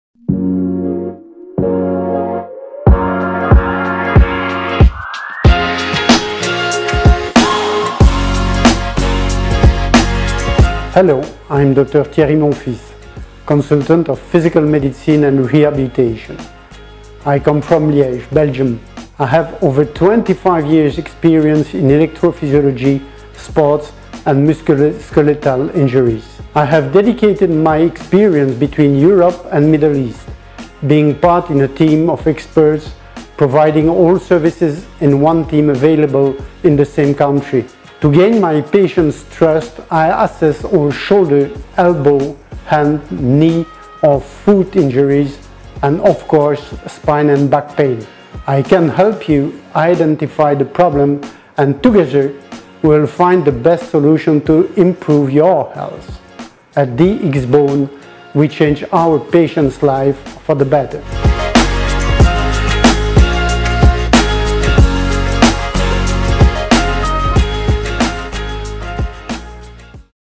Thierry Monfils, MD, Consultant, Physical Medicine and RehabilitationSpine Specialist
Having a special interest on musculoskeletal disorders and exploration of peripheral neuropathies, Dr Thierry Monfils has dedicated over 35 years of helping people regain function and improve quality of life.
know about Dr Thierry.